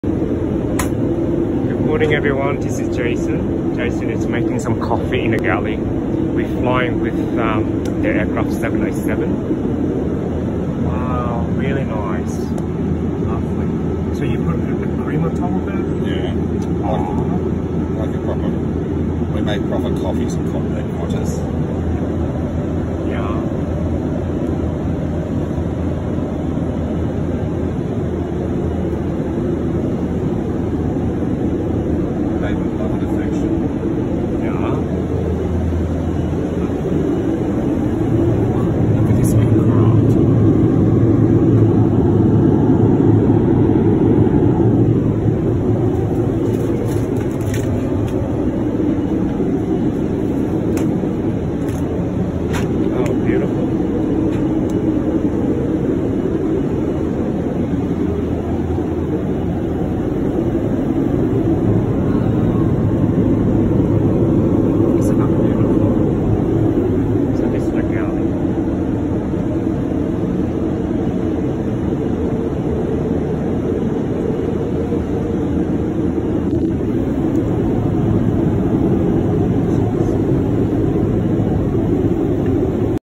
Qantas serves café style coffee in business class 🇦🇺🥰
Amazing coffees on a Qantas international flights ✈️🇦🇺🥰